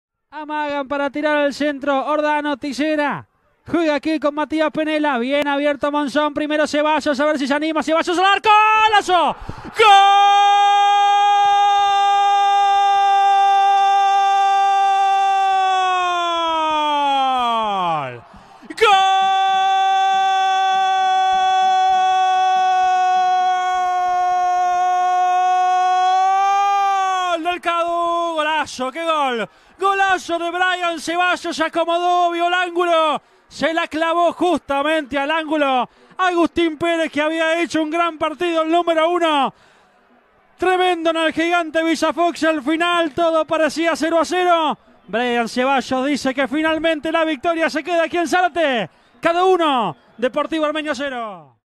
Primera B : DEFENSORES UNIDOS 1 - 0 DEPORTIVO ARMENIO | (El Gol)
PRIMERA B - Clausura 2020
Fecha 1
DEF: 45' (+4) (st) Ceballos
ARM:
¡¡¡ VOLVIÓ EL AMADO ASCENSO, VOLVIMOS!!!